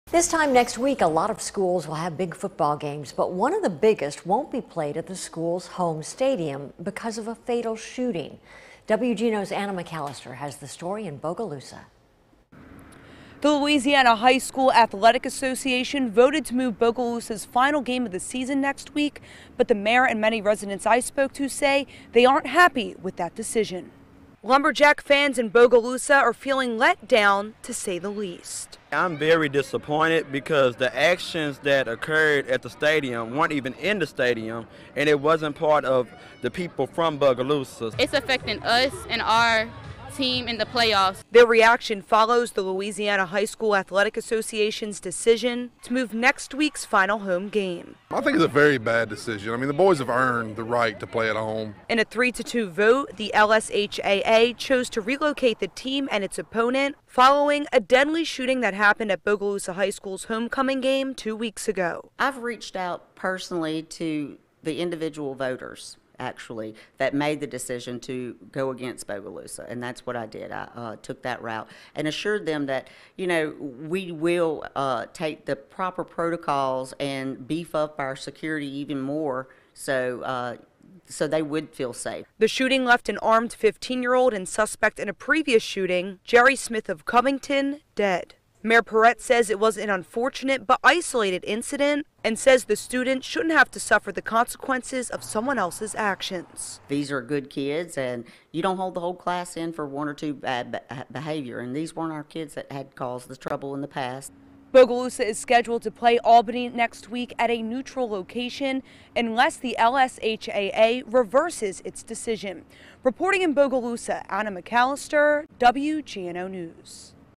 Bogalusa officials choosing to final home game
After a deadly shooting a week prior, Bogalusa officials are choosing the final home game of the season to a safer location.